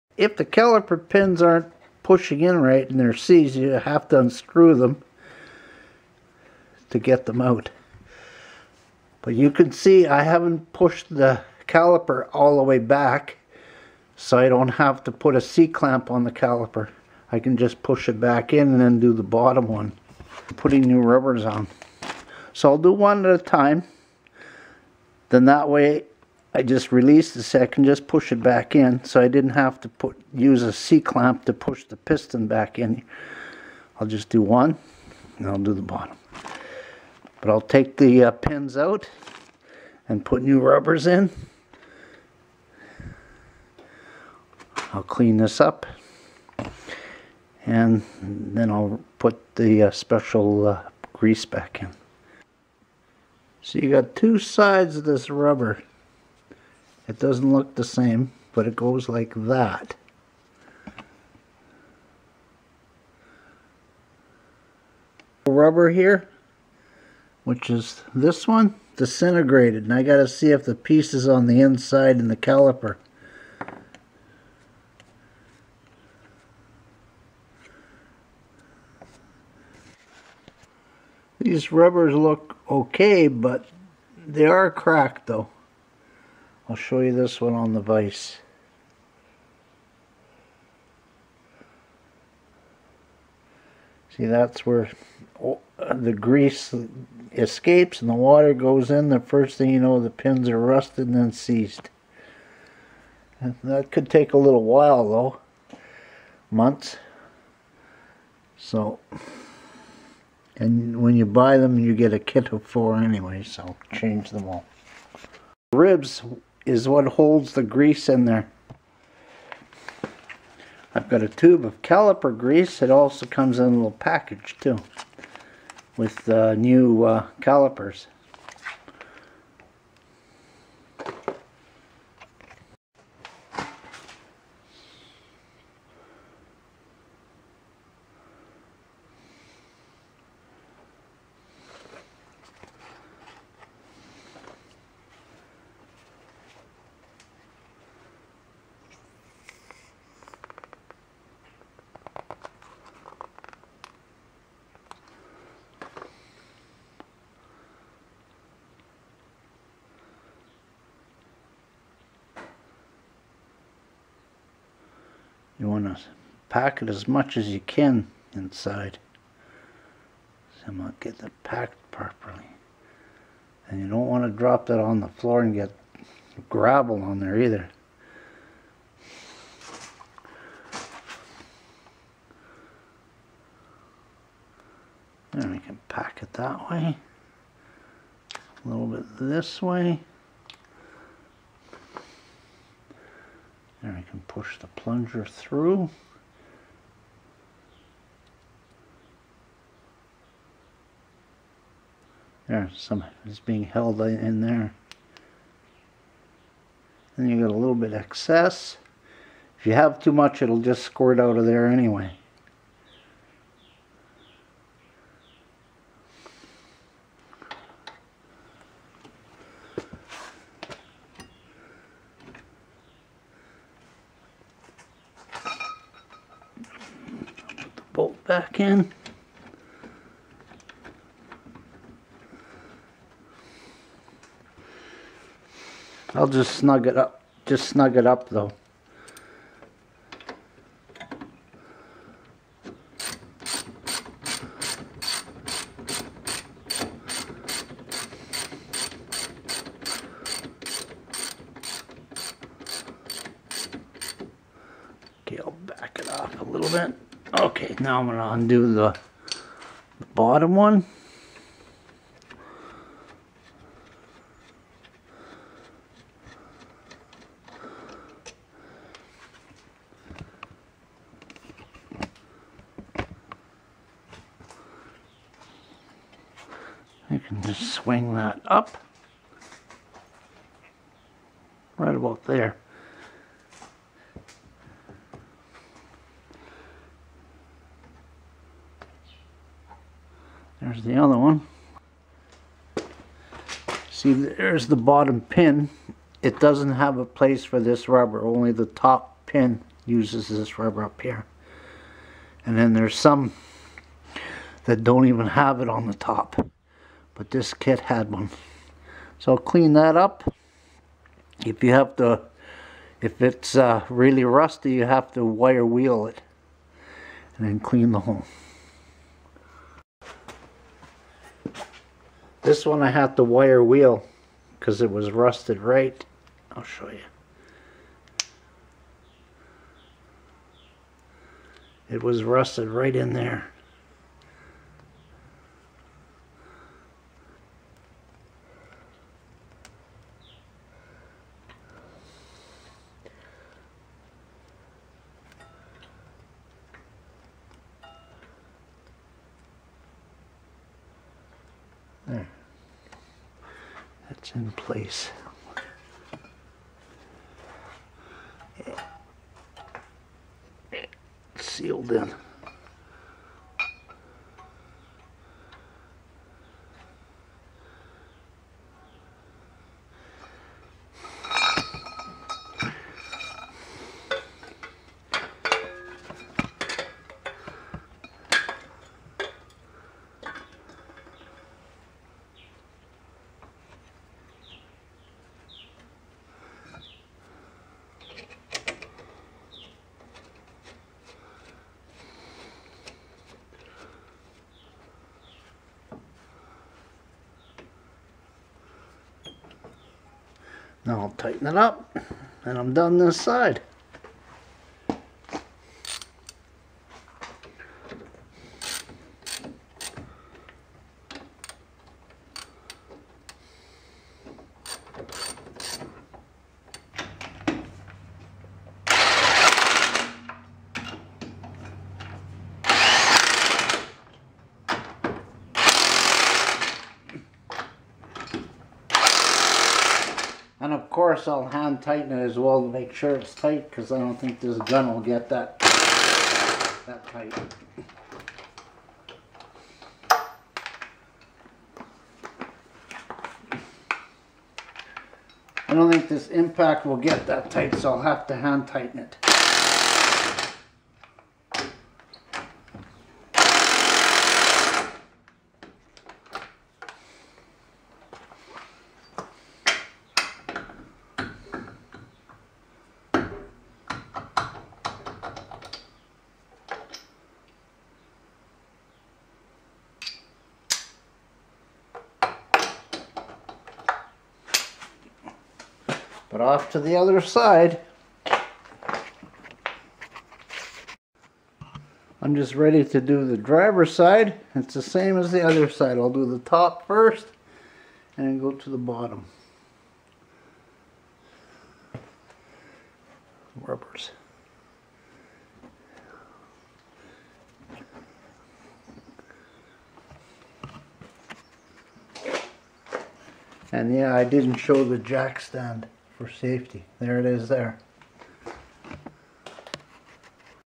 Caliper Pin Rubber Sleeve
Clean pins and replace with new rubber boots
when boots are old they crack and grease will escape and water will enter causing pins to seize.
This fixed my brakes from sticking as I had 2 seized pins